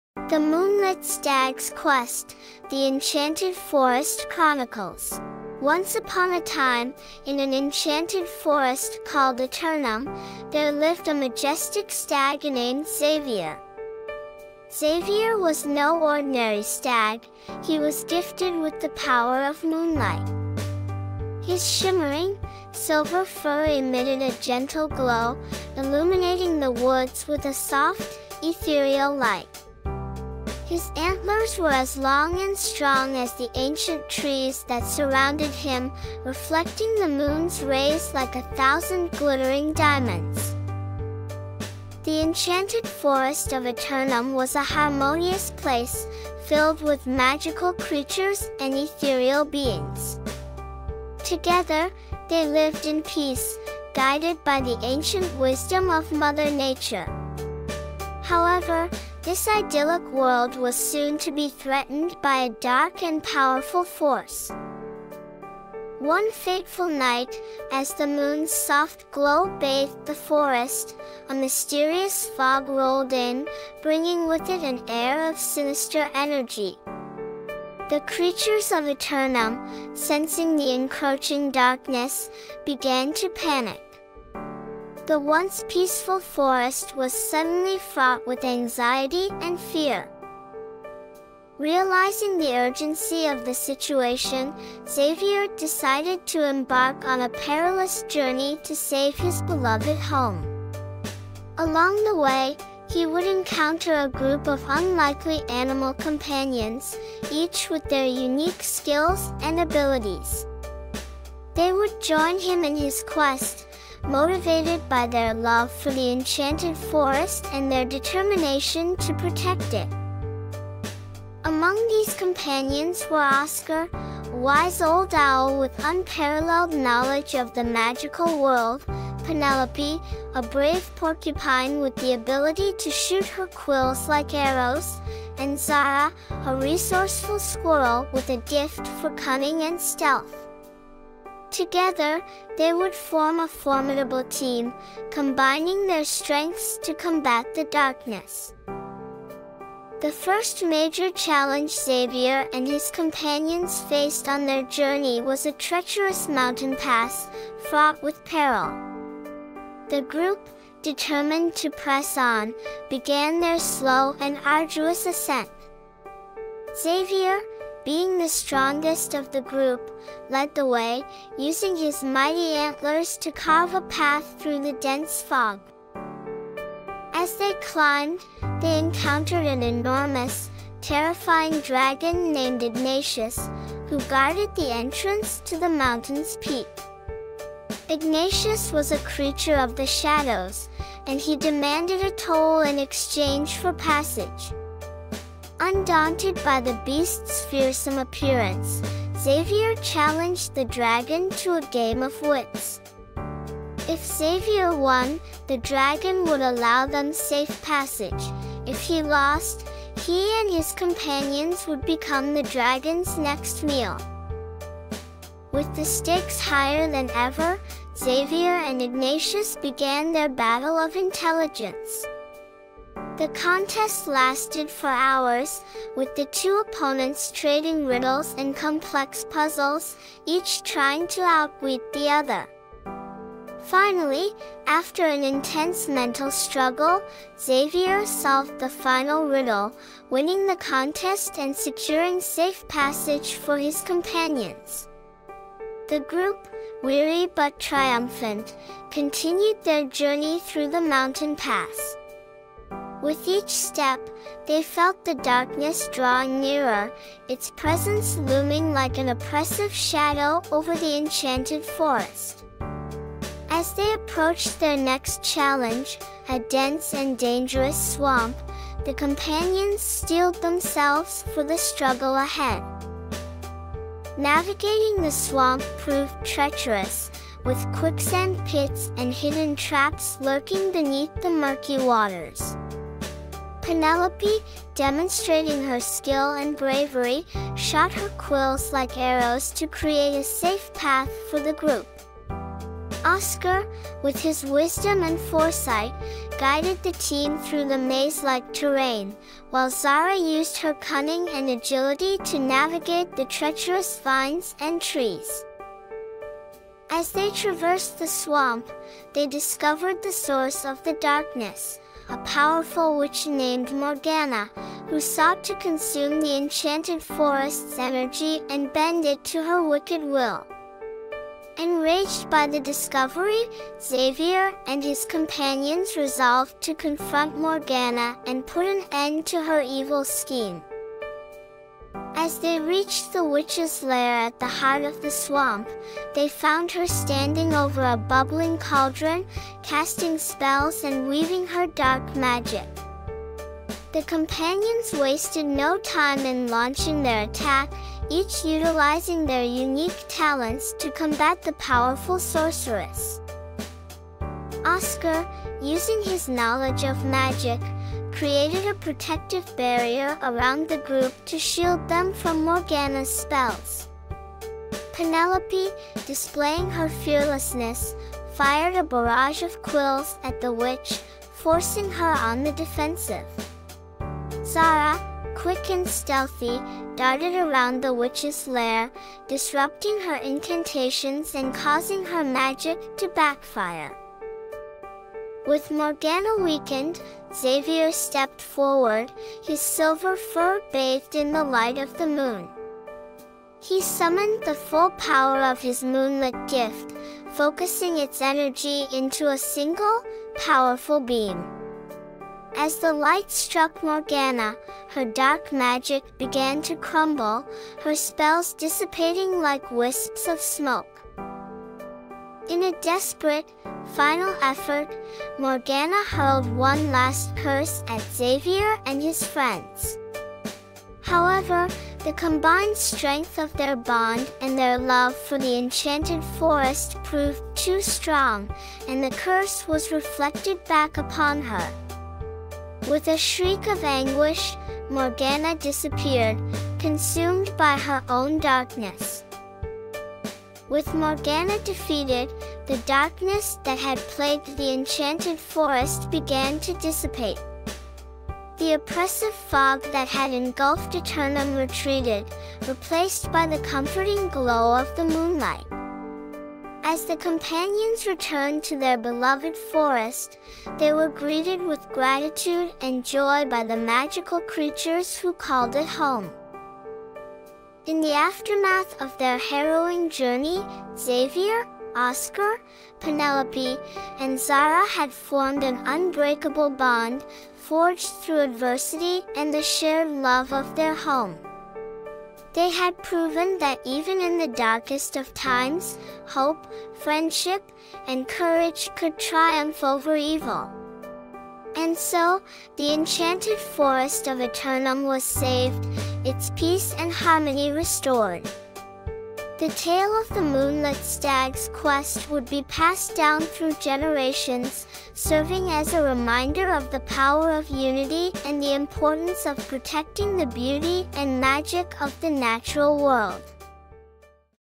Fairy Tales Audiobook - The Moonlit Stag's Quest: The Enchanted Forest Chronicles
Embark on an unforgettable journey with Xavier, the Moonlit Stag, and his unlikely band of animal companions as they venture through the magical world of Eternum. When a mysterious darkness threatens their enchanted forest, they must overcome fearsome challenges, confront a powerful witch, and discover the true strength of friendship. The Moonlit Stag's Quest: The Enchanted Forest Chronicles is a spellbinding tale that will captivate readers of all ages and remind them of the power of hope, courage, and unity.